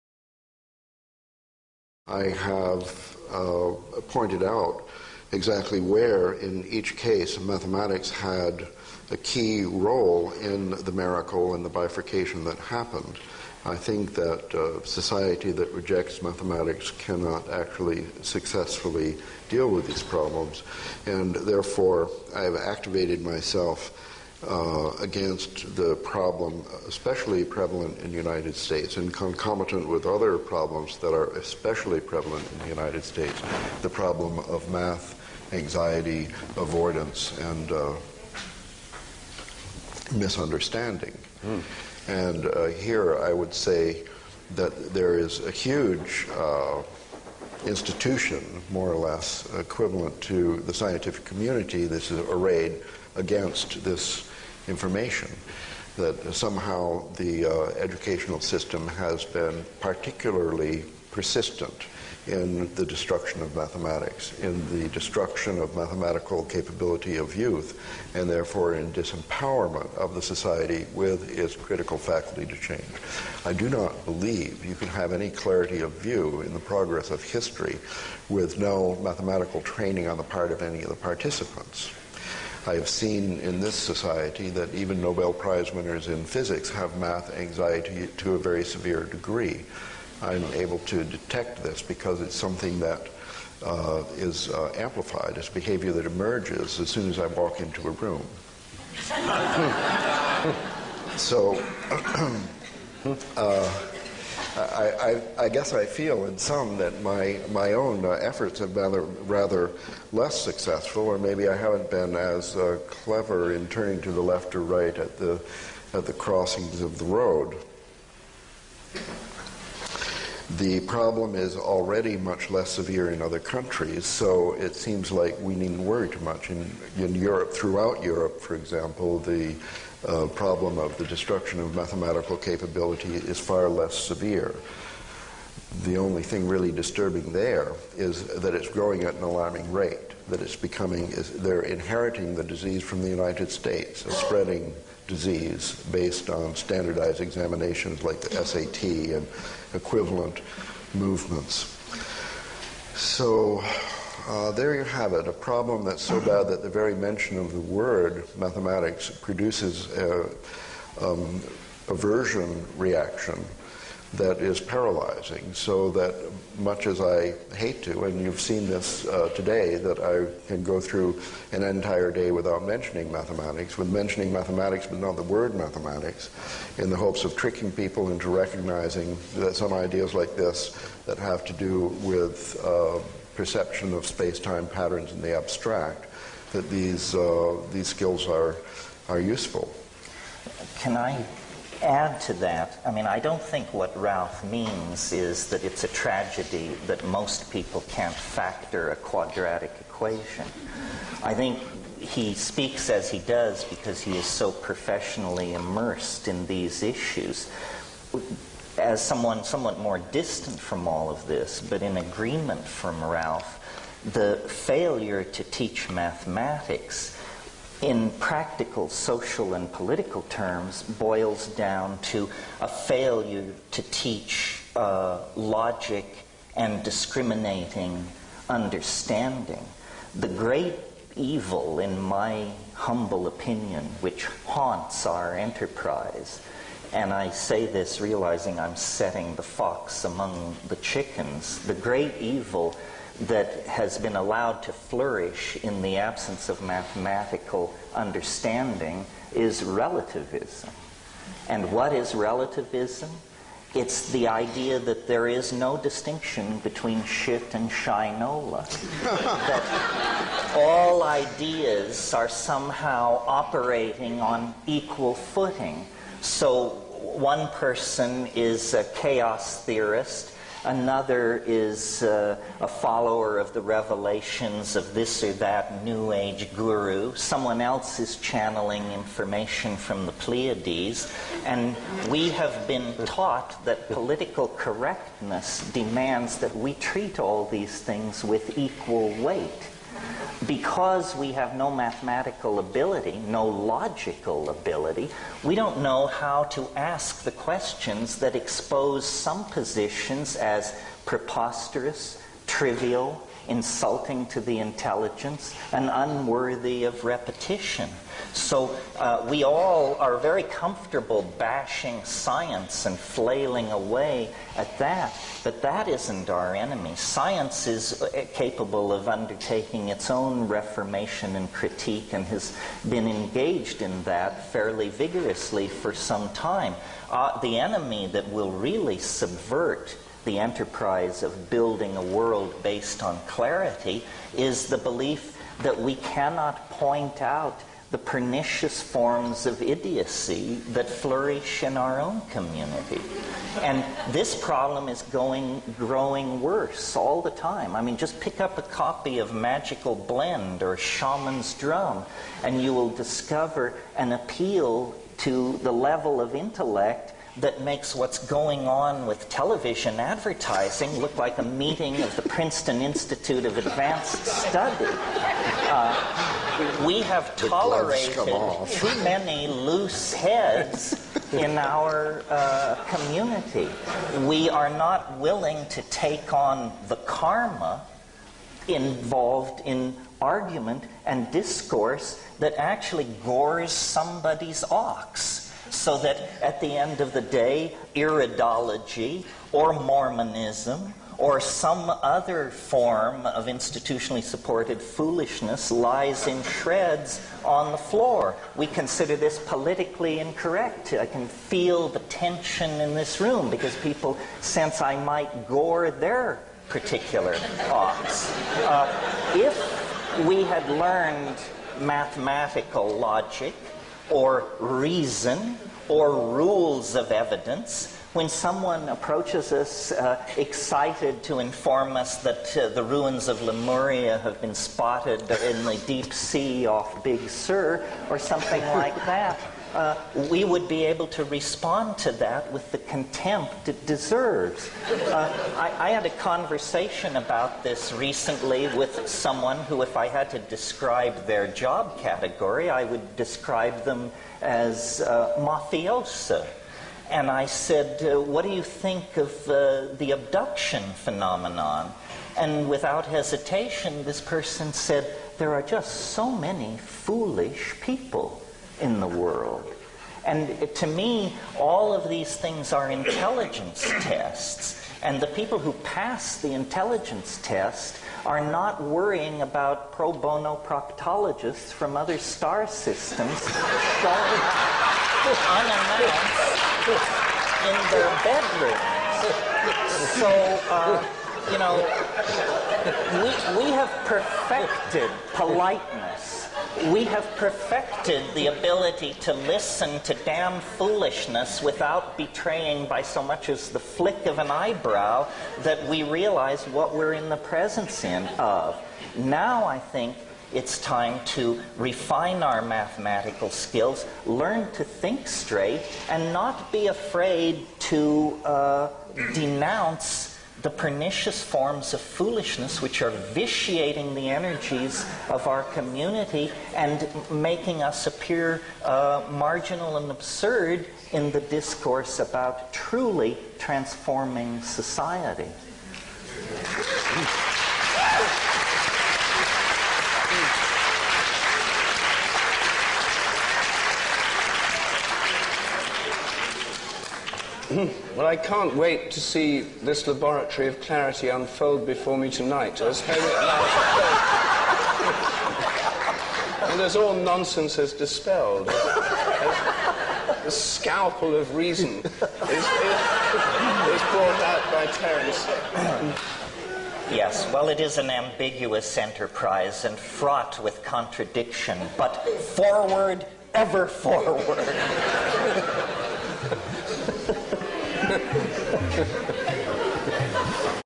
Ralph Abraham gets wordsmithed by Terence Mckenna
Both brilliant minds, but I think it's obvious who is the better wordsmith of the two :)
About the problem of lack of mathematical and logical education making it hard for the general public to make informed decisions about anything, from life to politics. Ralph gives a mathematicians perspective, Mckenna breaks it down by weaving his words with the usual surgical precision.